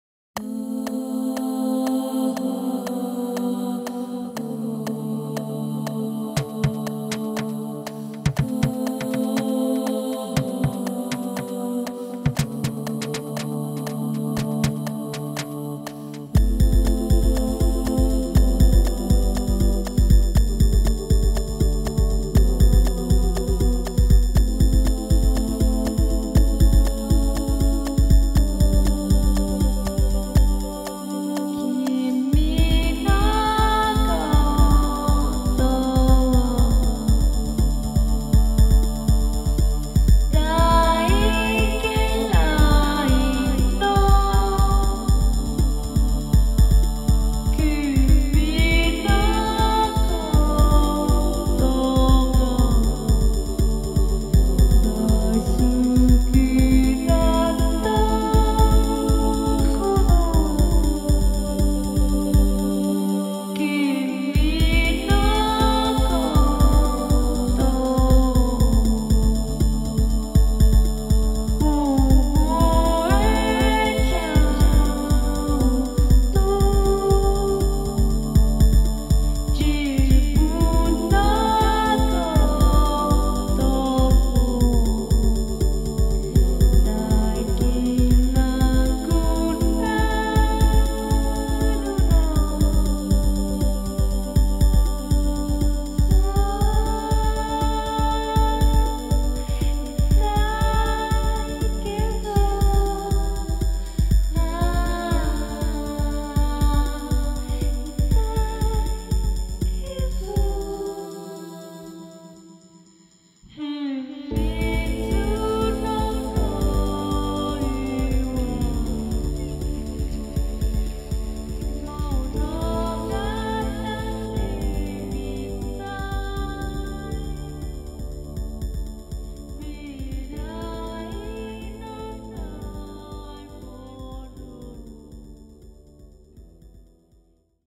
beach boy
Saturday at the leash-free beach with Rufus the Red.
FMA Creative Commons music by N. Hayakawa.